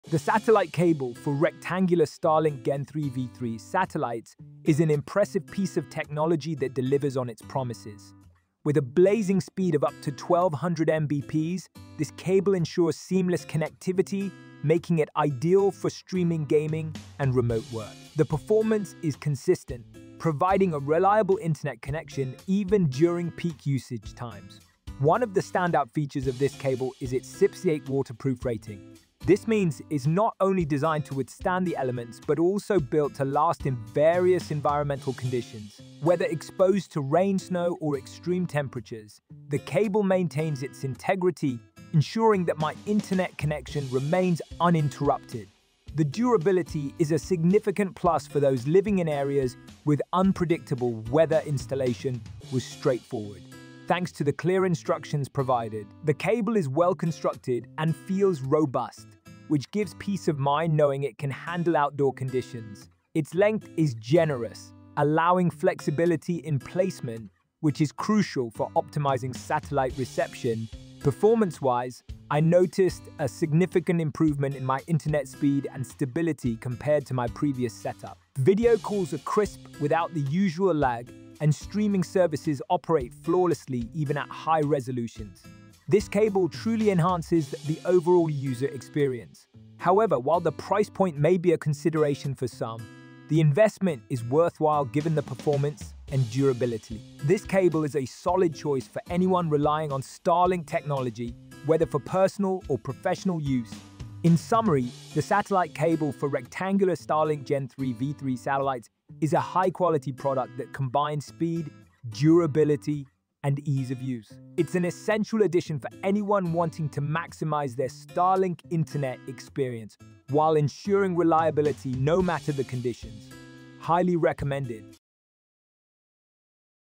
Satellite Cable for Rectangular Starlink Gen3 V3 Satellites 1200 Mbps IP68 Waterproof Review
The Satellite Cable for Rectangular Starlink Gen3 V3 Satellites is an impressive piece of technology that delivers on its promises. With a blazing speed of up to 1200 Mbps, this cable ensures seamless connectivity, making it ideal for streaming, gaming, and remote work. The performance is consistent, providing a reliable internet connection even during peak usage times.

One of the standout features of this cable is its IP68 waterproof rating. This means it’s not only designed to withstand the elements but also built to last in various environmental conditions. Whether exposed to rain, snow, or extreme temperatures, the cable maintains its integrity, ensuring that my internet connection remains uninterrupted. The durability is a significant plus for those living in areas with unpredictable weather.

Installation was straightforward, thanks to the clear instructions provided. The cable is well-constructed and feels robust, which gives peace of mind knowing it can handle outdoor conditions. Its length is generous, allowing flexibility in placement, which is crucial for optimizing satellite reception.

Performance-wise, I noticed a significant improvement in my internet speed and stability compared to my previous setup. Video calls are crisp, without the usual lag, and streaming services operate flawlessly even at high resolutions. This cable truly enhances the overall user experience.

However, while the price point may be a consideration for some, the investment is worthwhile given the performance and durability. This cable is a solid choice for anyone relying on Starlink technology, whether for personal or professional use.

In summary, the Satellite Cable for Rectangular Starlink Gen3 V3 Satellites is a high-quality product that combines speed, durability, and ease of use. It’s an essential addition for anyone wanting to maximize their Starlink internet experience while ensuring reliability no matter the conditions. Highly recommended!